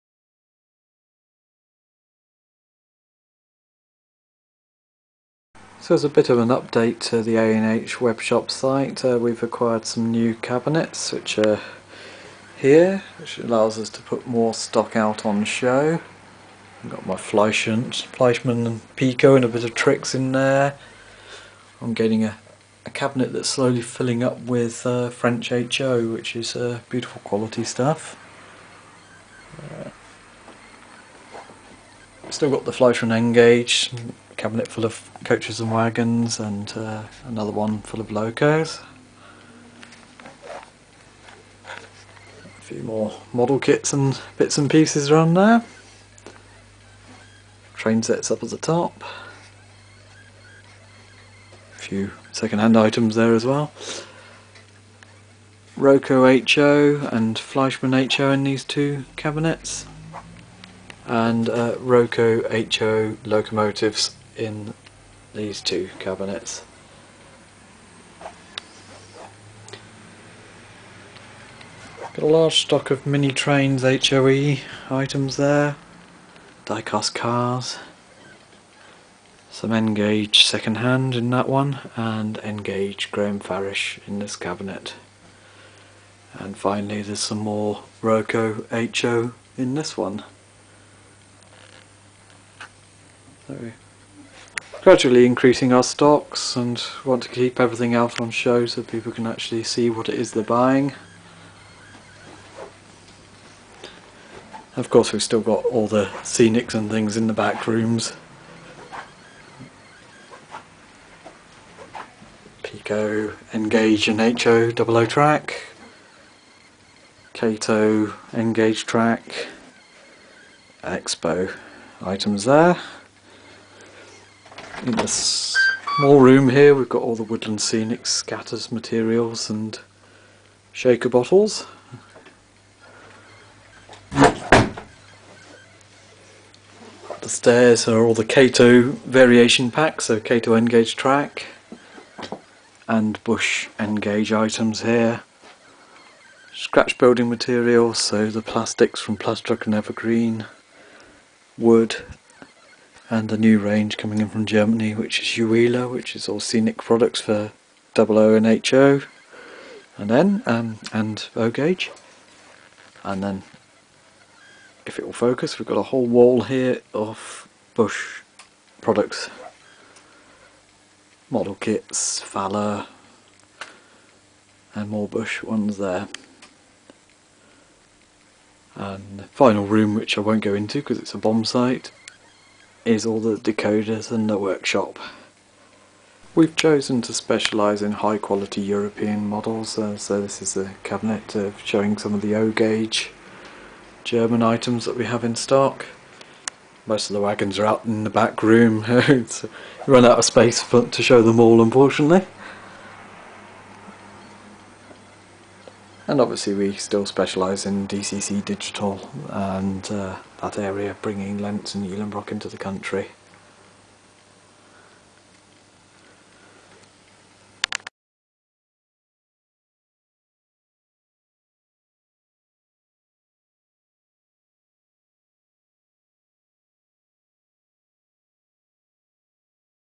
Shop update film
A short new film to show the new layout of the shop.  January 2015.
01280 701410